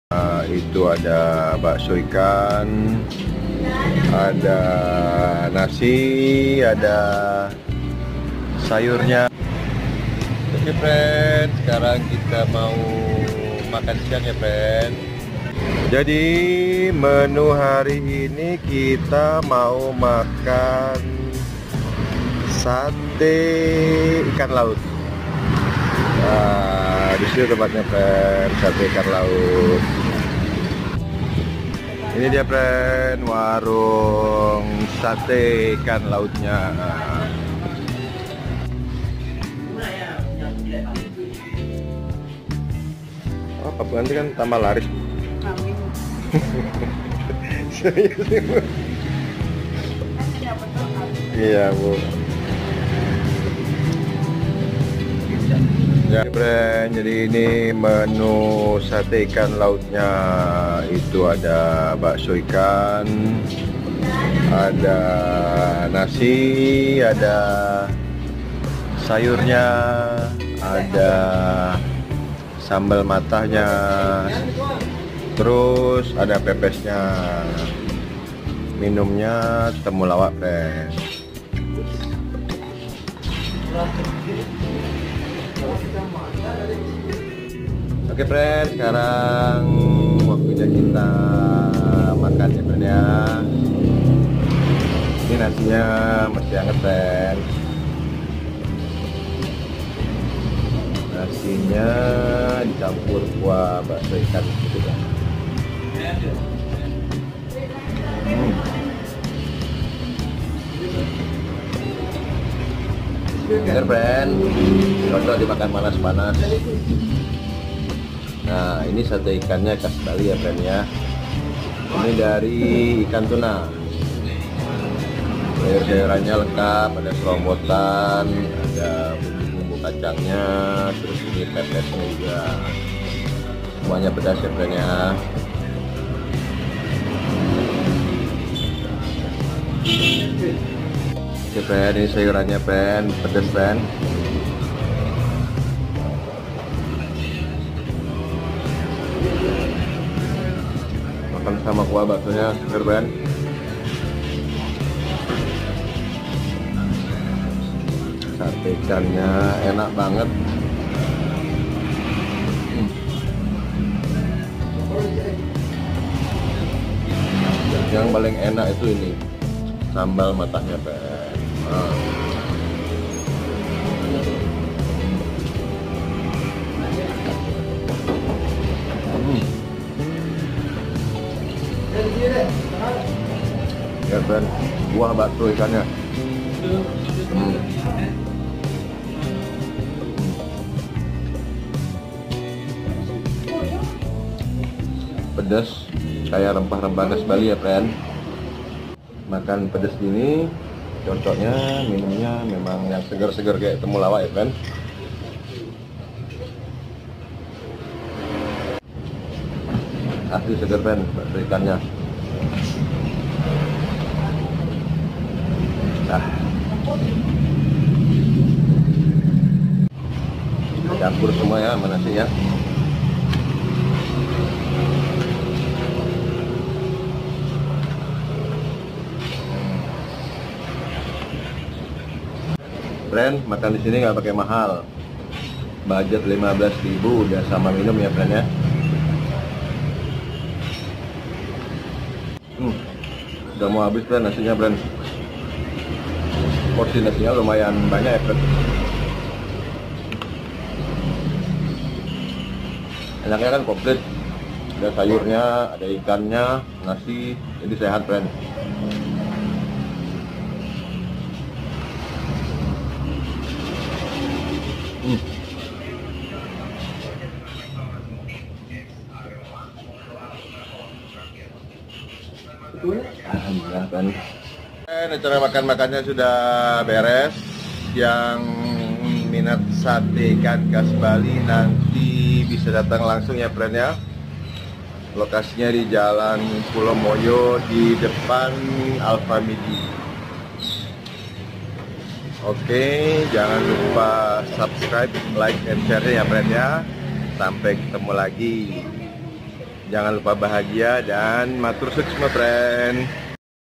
Sate Ikan paling naamppooolll sehari 5000 porsi wwoooww..!!!
Oke Pren waktunya makan siang berati waktunya Bali Happy Centre Kuliner bareng saya Fadil Bali Happy Centre ya Pren..

Siang gini pas makan Sate Ikan Laut komplit khas Bali, seporsi termasuk Nasi, Sate Ikan, Sayuran, Pepes Ikan & Bakso Ikan cuma 10.000 an..

Tambah Es Temulawak cuma 16.000 Pren...